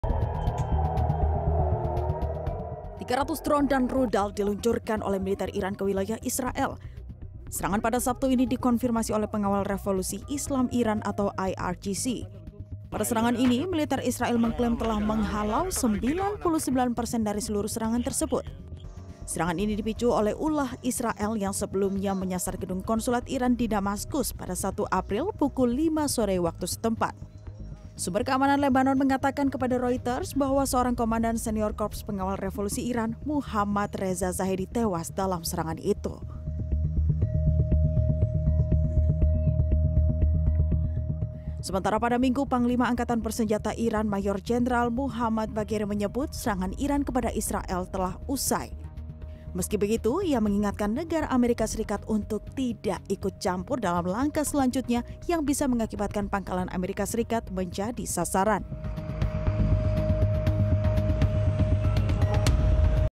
Iran Sebut Serangan ke Israel Telah Usai, Ingatkan Negara Lain Tak Ikut Campur
Iran melancarkan serangan drone ke israel pada sabtu lalu. Serangan Iran ini sebagai balasan, setelah awal april lalu Israel menyerang gedung konsulat iran di damaskus, suriah.

Militer Iran menyebut serangan ini telah selesai. Namun mereka mengingatkan negara lain, untuk tidak ikut campur dalam langkah selanjutnya.